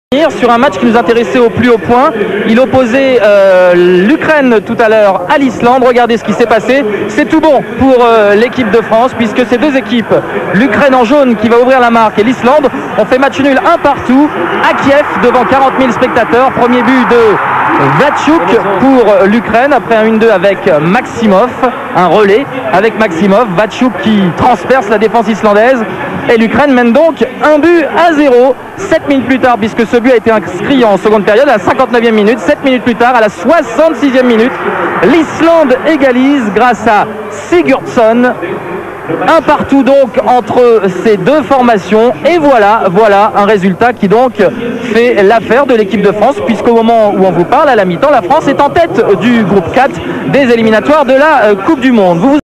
1999 - Україна - Ісландія. Ukraine - Iceland. 1:1 (голи)
Україна - Ісландія - 1:1 (матч збірної України №50).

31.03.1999. Київ. Стадіон НСК "Олімпійський". 50000 глядачів.
Відбірковий турнір ЧЄ-2000. Група 4. 5 тур.

Голи:
Ващук Владислав (Максимов Юрій), 59 (1:0)
Larus Sigurdsson (Thordur Gudjonsson), 66 (1:1)